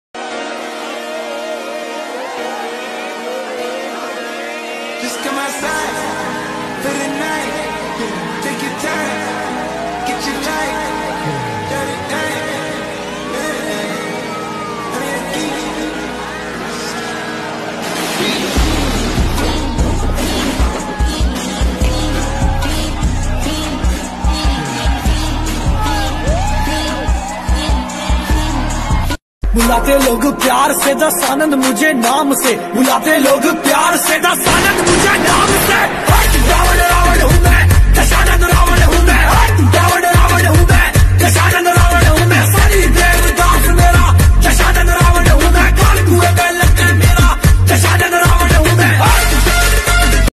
Dussehra is incomplete without these Viral videos! When Ravan Dahan went Wrong-Watch
The entire country is drenched in the festive fervour of Dussehra or Vijayadasami. As per tradition, Goddess Durga's idols are worshipped across the country on pandals and Demon King Ravana's effigy is burned to celebrate the victory of Lord Ram. Here we bring two viral videos of Ravan Dahan that will tickle your funny bones.